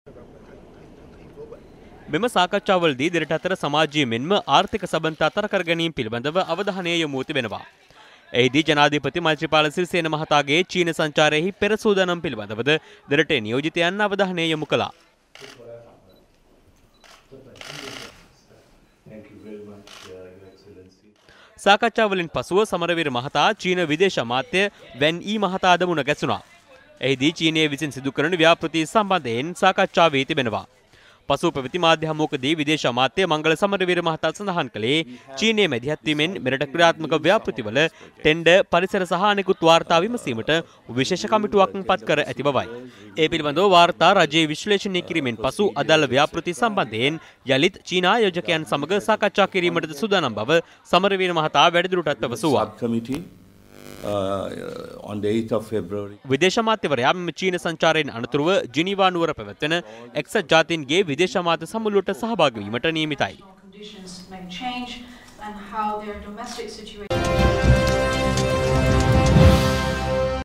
Mangala meets Chinese counterpart during Beijing visit
Sri Lankan Foreign Minister Mangala Samaraweera met with China’s Premier Li Keqiang and his Chinese counterpart during his visit to Beijing.